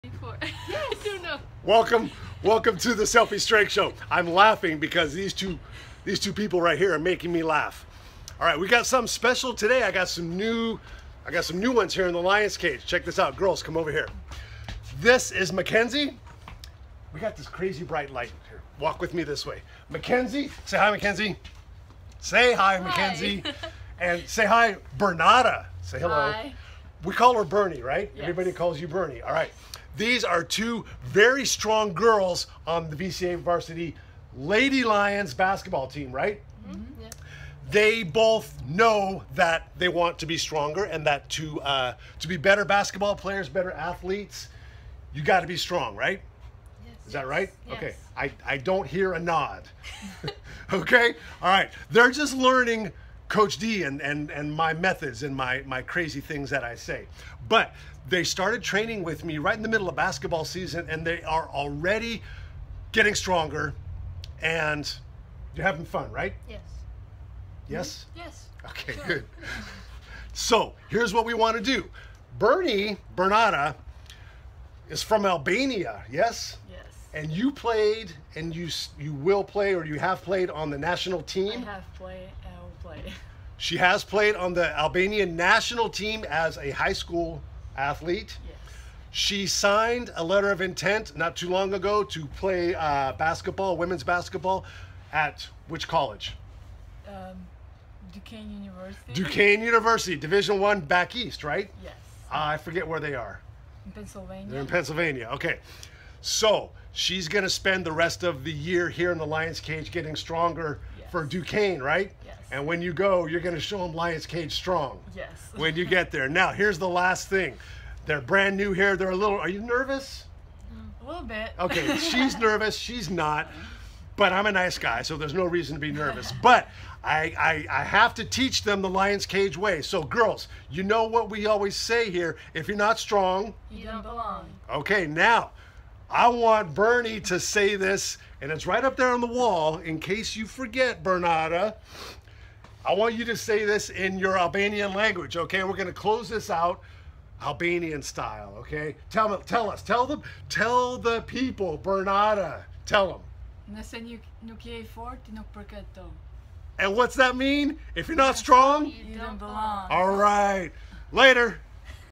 Can you say, "If You're Not Strong You Don't Belong" - in Albanian??
1/4/18

Coach D learns how to speak Albanian!